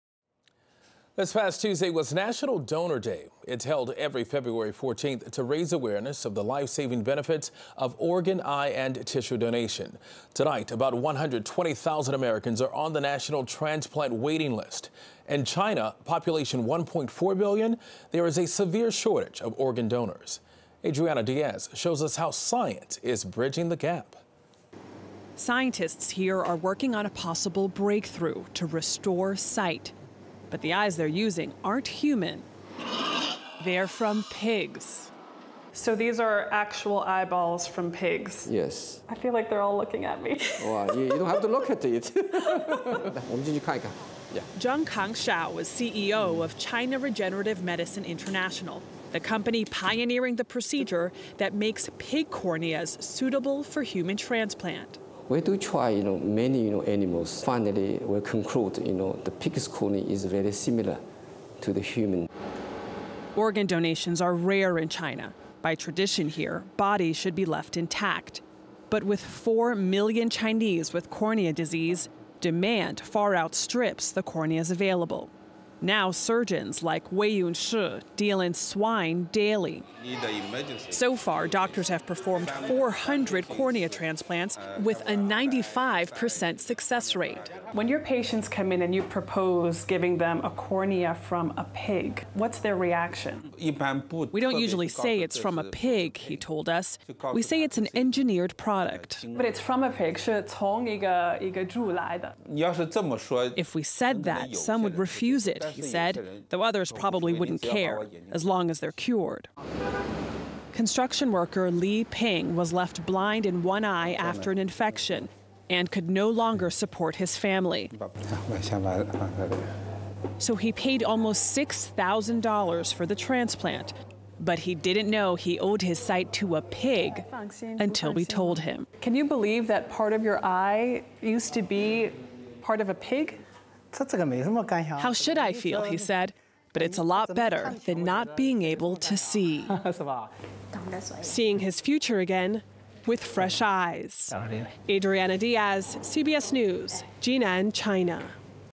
Scientists working on restoring human sight with pig eyes
National Donor Day is held every February to raise awareness of the lifesaving benifits of organ, eye and tissue donation. In China, where the population is 1.4 billion, scientists are working on a procedure that makes pig corneas suitable for human transplant. Adriana Diaz has the story.